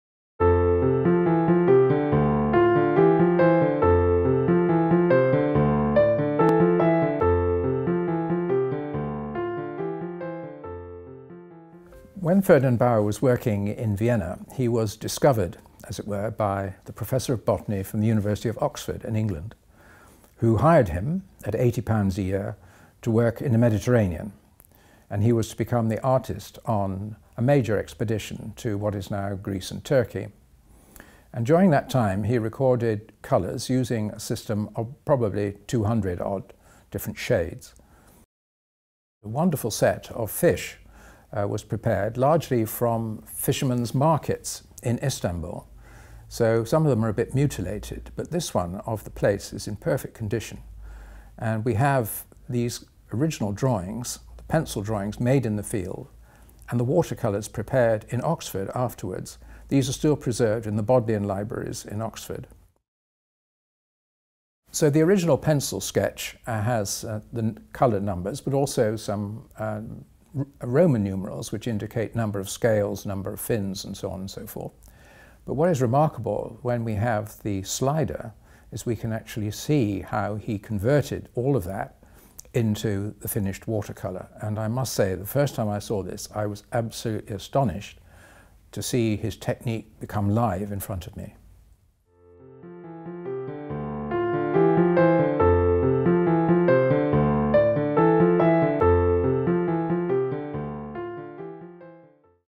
Pleuronectes platessa
A video from the online experience Painting By Numbers

Pleuronectes platessa (Scientific Name) by Ferdinand Bauer. Image The Bodleian Library, University of Oxford. Common name European Plaice.

Ferdinand Lucas Bauer (20 January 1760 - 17 March 1826) an Austrian botanical illustrator is widely held to be the greatest natural history painter of all time – and certainly the greatest to have worked in Australia. His work of biological illustration melds scientific accuracy with artistic verve.